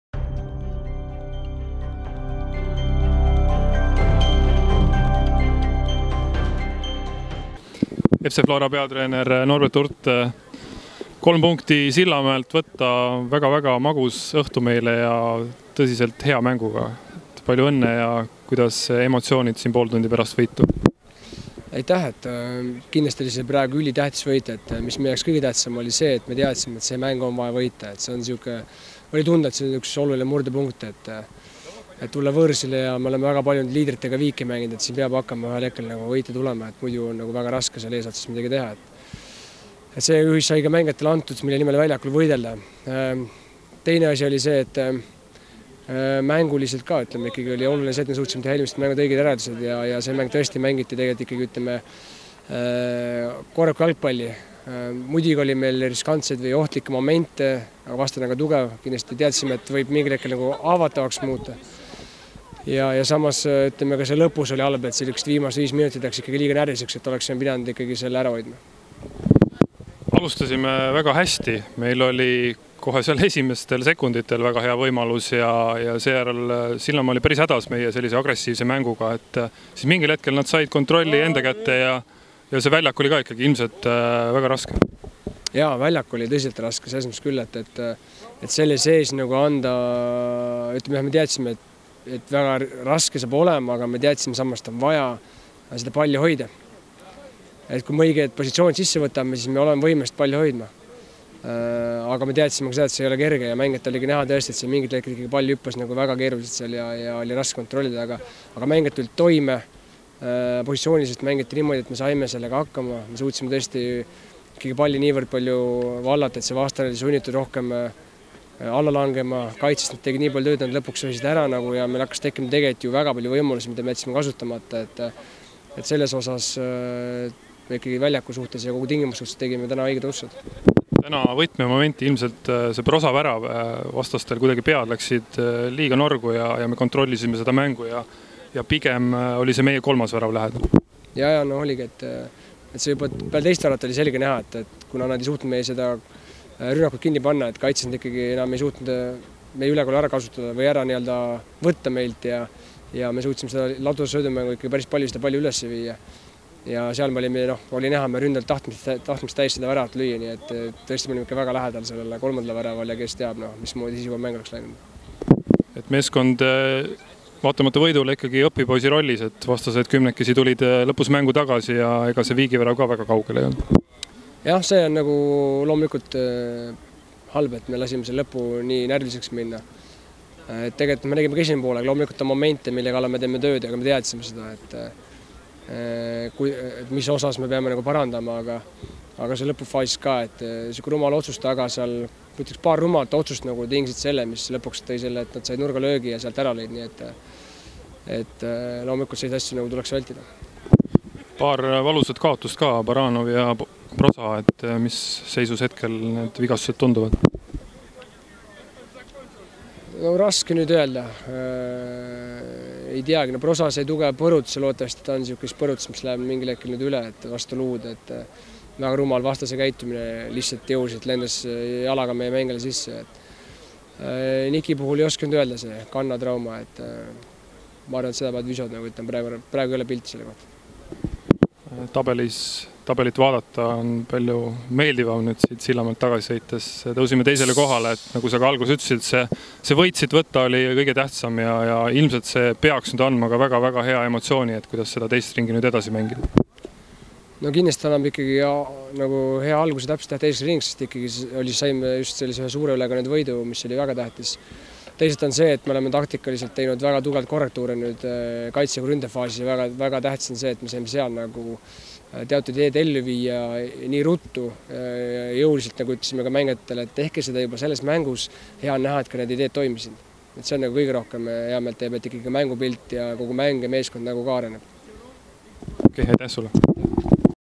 TV FLORA: Norbert Hurt: ülitähtis võit!
FC Flora sai Premium liiga 10. voorus Sillamäel tulemusega 2:1 jagu sealsest Kalevist.